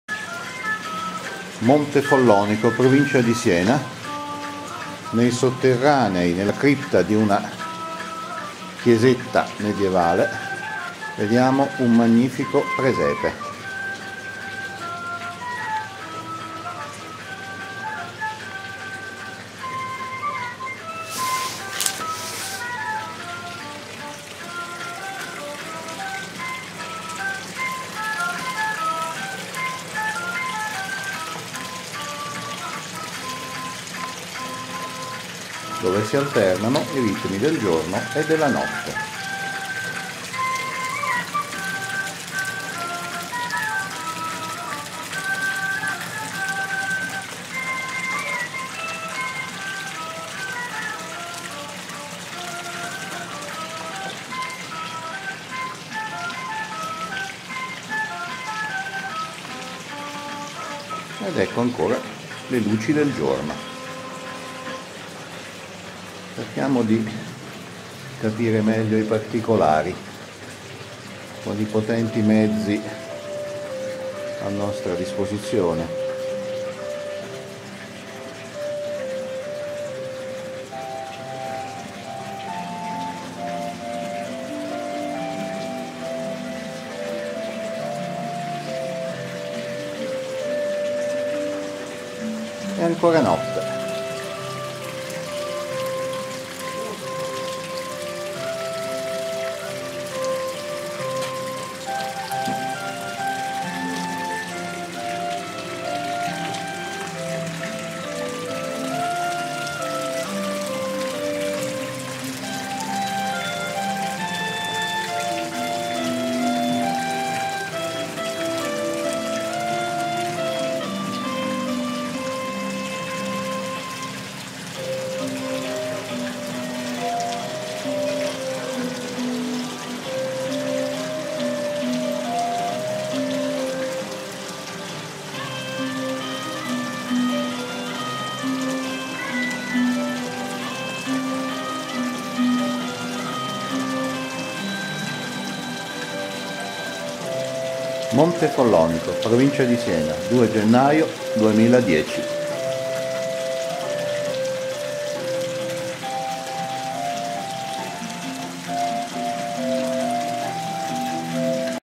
Presepio a Montefollonico (Siena).
Semplice ma completo e gradevolissimo il presepio situato all´interno della cripta di una antica chiesetta esattamente all´interno delle mura di Montefollonico (SI) piccolo centro interamente medievale, frazione del Comune di Torrita di Siena ed a pochi km da Pienza.